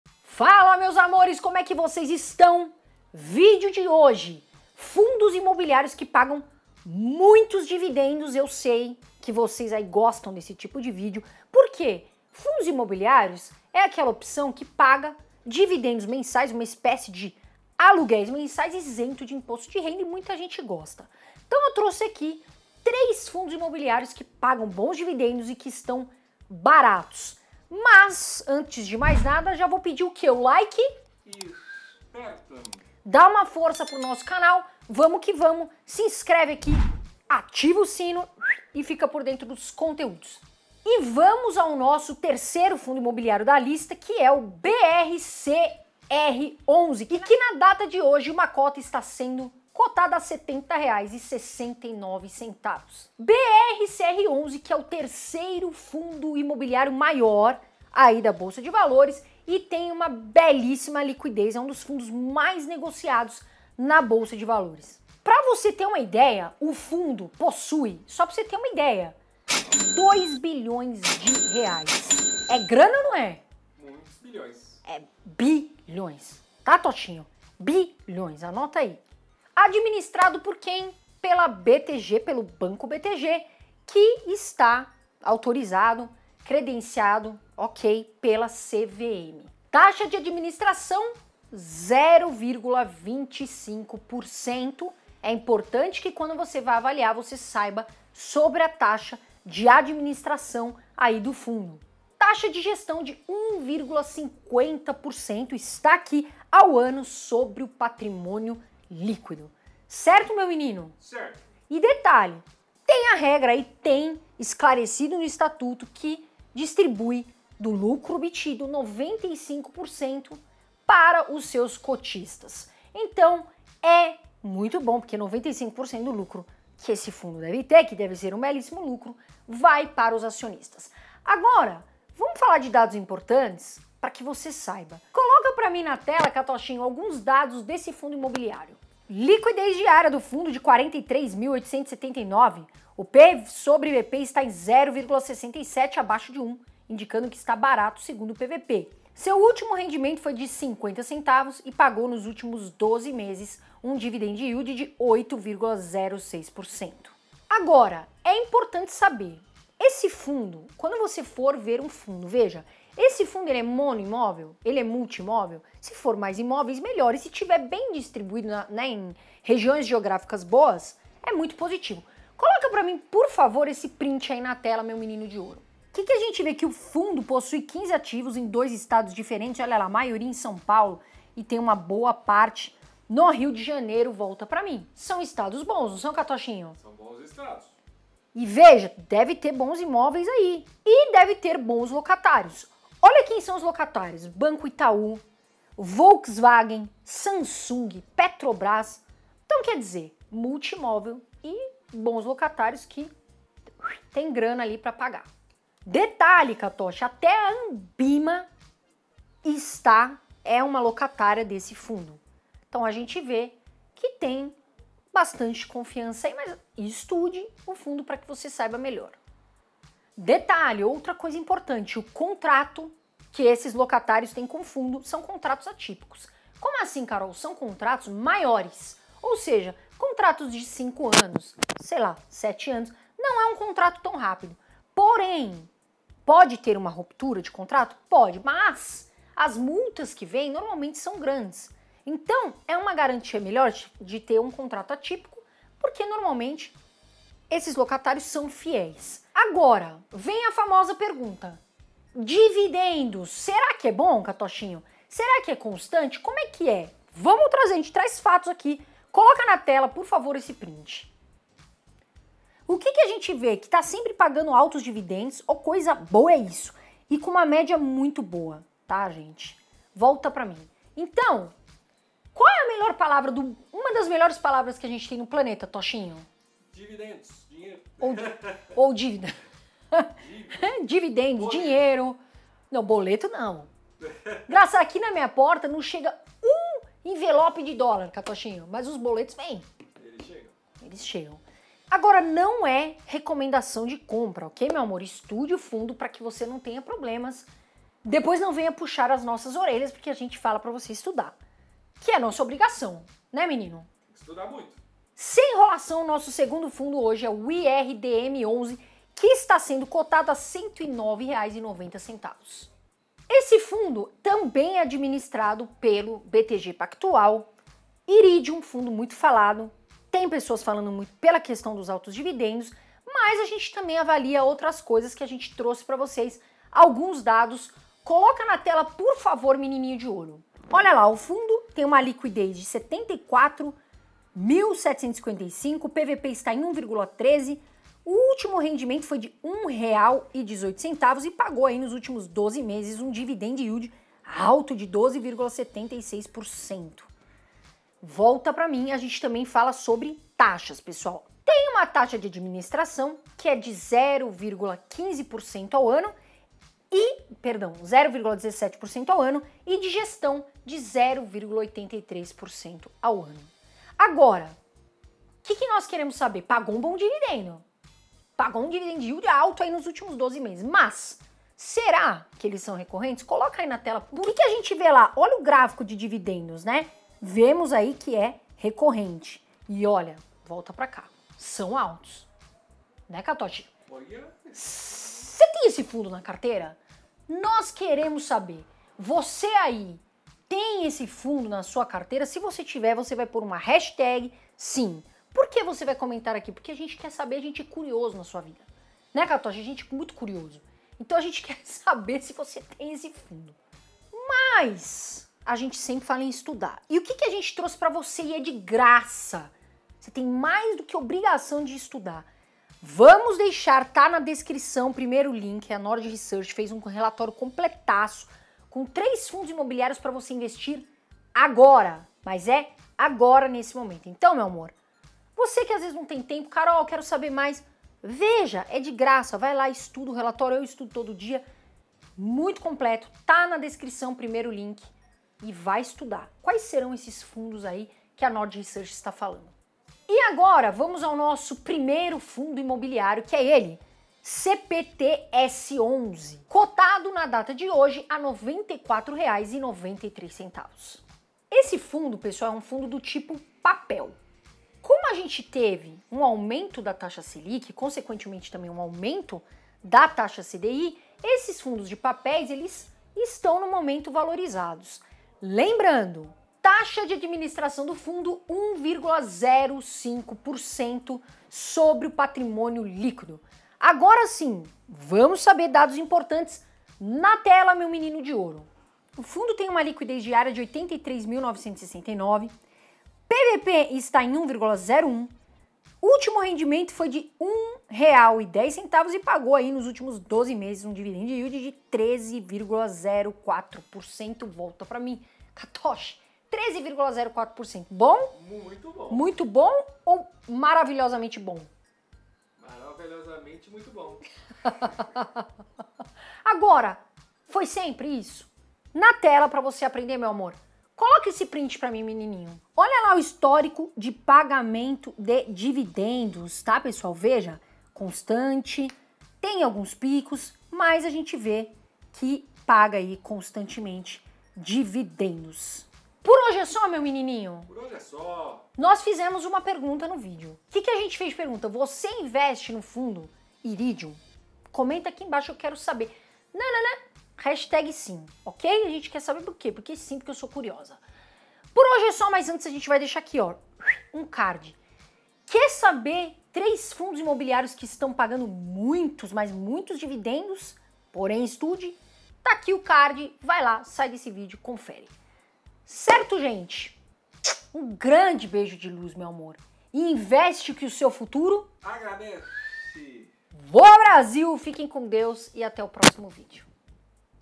3 MELHORES FUNDOS IMOBILIARIOS QUE PAGAM BONS DIVIDENDOS para 2022
📱 Relatório gratuito- 3 FIIS PARA INVESTIR AGORA EM 2022

Você quer conhecer Fundos Imobiliários baratos, que pagam bons dividendos? Esse é o tema do nosso vídeo de hoje.

Nesse vídeo você vai ver:

00:00​ Tema do vídeo
00:44 3° Fii
05:08 2° FII
07:20 1° FII